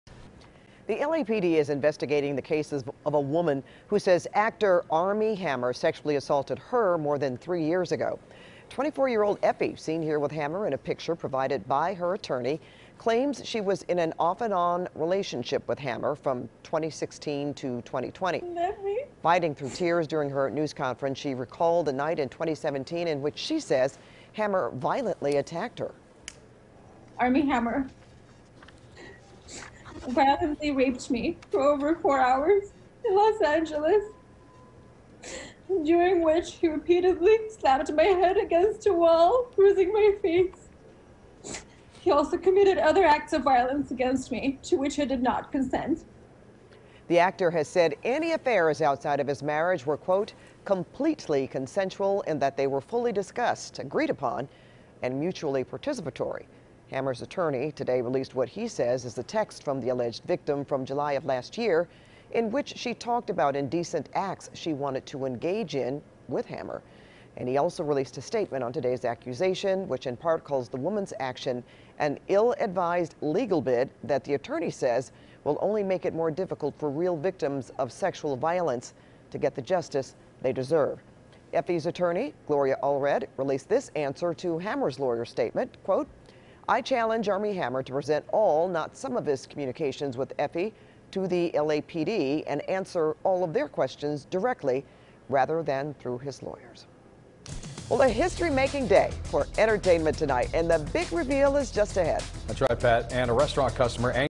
Woman Alleges Actor Armie Hammer Raped Her, LAPD Investigating
A woman who alleges she was sexually assaulted by actor Armie Hammer held a news conference Thursday alongside celebrity attorney Gloria Allred.